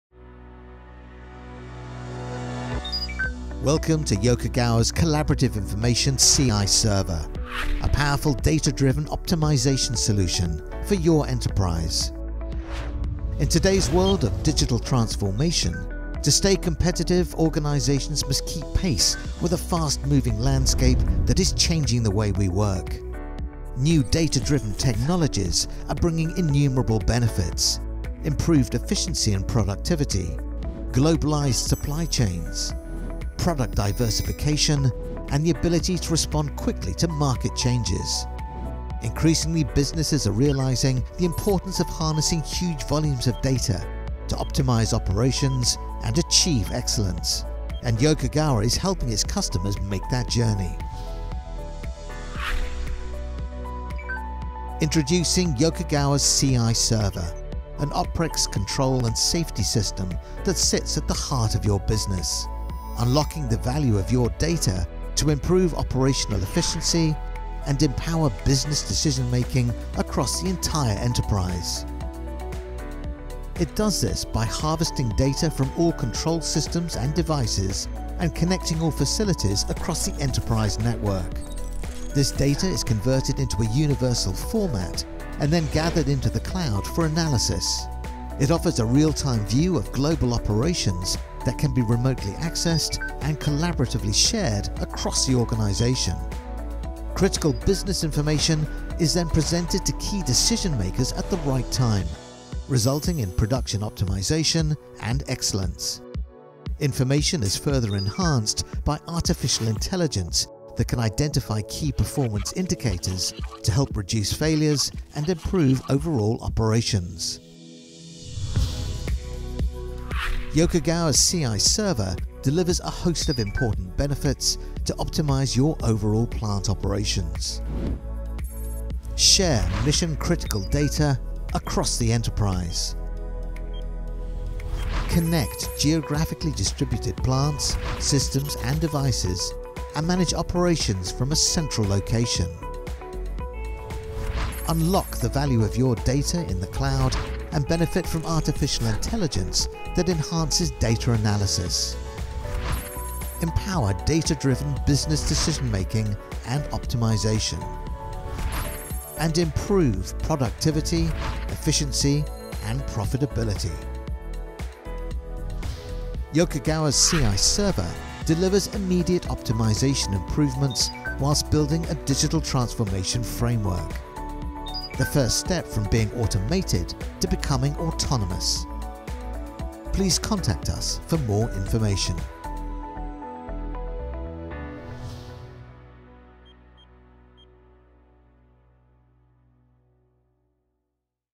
Collaborative Information (CI) Server- improve production efficiency through Digital Transformation
Plants are often scattered among remote base sites. These sites can contain multiple plants, each with their own array of equipment and devices, utilizing various systems to carry out management and operations tasks according to their respective functionality. In turn, each piece of equipment and system may connect to a variety of devices.

Yokogawa's Collaborative Information Server integrates this complex hierarchy to unlock critical process data required to achieve data-driven optimization. This allows immediate improvement to production efficiency through DX (digital transformation) and reduction of operational maintenance whilst building a digital transformation framework.